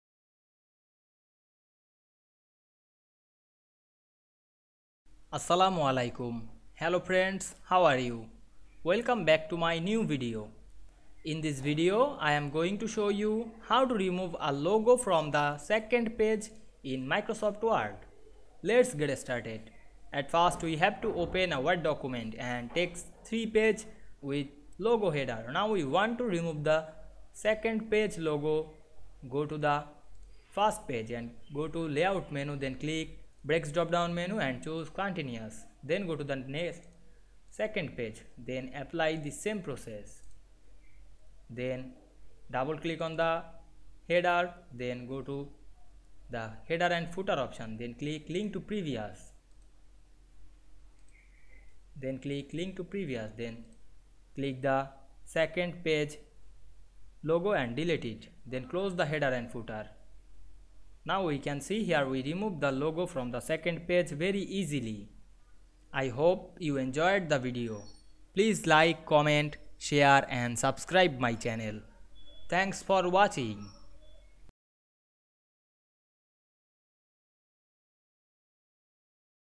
How to remove a logo from the second page in Word
Assalamu Walaikum,
In this video I will show you, How to remove a logo from the second page in Word. Let's get started.